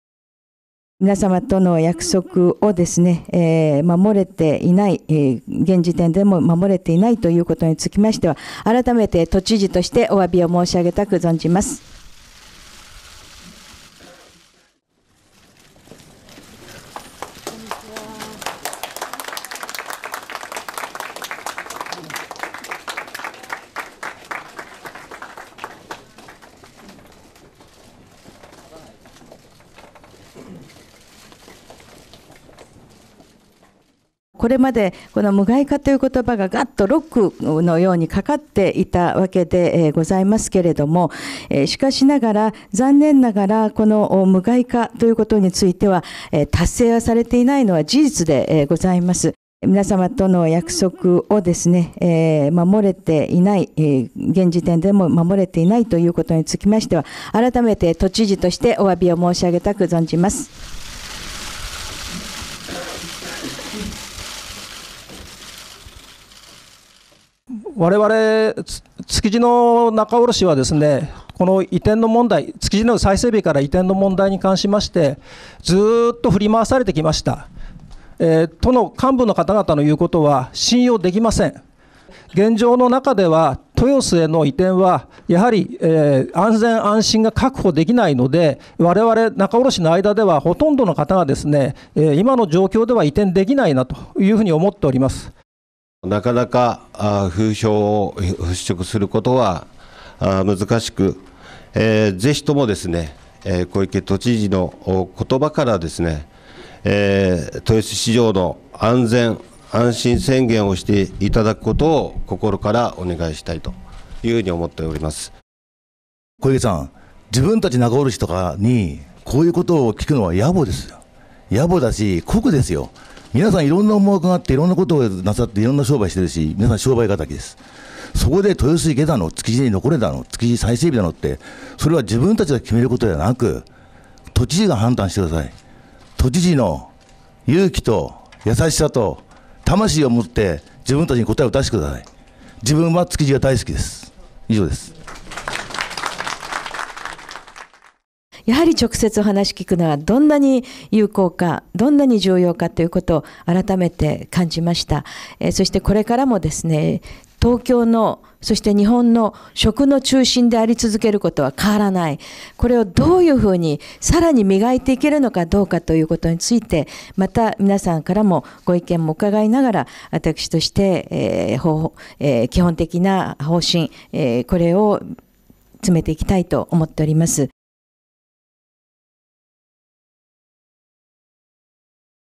小池知事、築地業者に陳謝 豊洲「無害化」未達成で
東京都の小池百合子知事は１７日、築地市場（中央区）を訪れ、豊洲市場の地下水などの有害物質を環境基準値以下にする「無害化」が達成できていない現状を「約束を守れていないことは、都知事としておわびする」と市場業者に陳謝した。